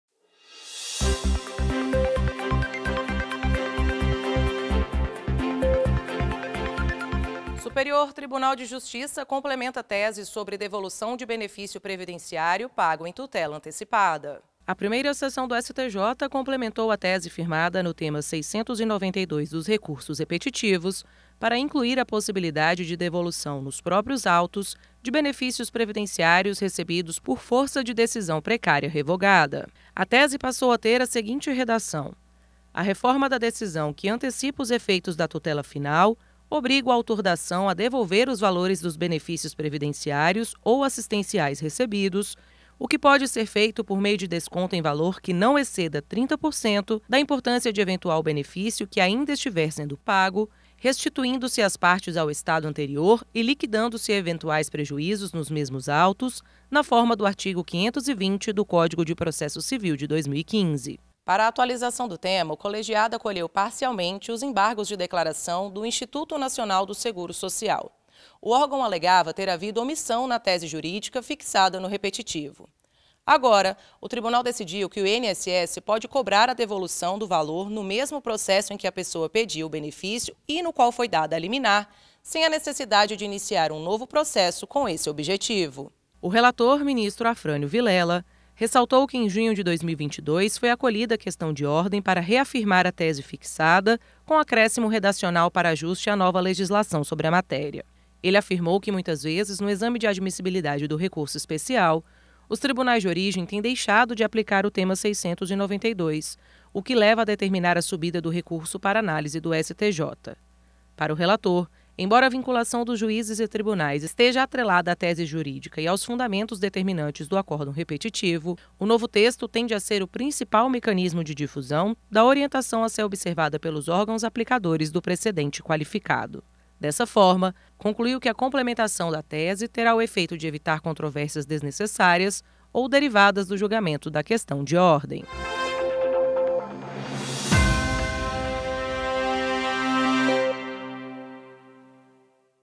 STJ complementa tese sobre devolução de benefício previdenciário pago em tutela antecipada
A Primeira Seção do Superior Tribunal de Justiça (STJ) complementou a tese firmada no Tema 692 dos recursos repetitivos para incluir a possibilidade de devolução nos próprios autos de benefícios previdenciários recebidos por força de decisão precária revogada.
A tese passou a ter a seguinte redação: "A reforma da decisão que antecipa os efeitos da tutela final obriga o autor da ação a devolver os valores dos benefícios previdenciários ou assistenciais recebidos, o que pode ser feito por meio de desconto em valor que não exceda 30% da importância de eventual benefício que ainda lhe estiver sendo pago, restituindo-se as partes ao estado anterior e liquidando-se eventuais prejuízos nos mesmos autos, na forma do artigo 520, II, do Código de Processo Civil de 2015 (artigo 475-O, II, do CPC/1973)".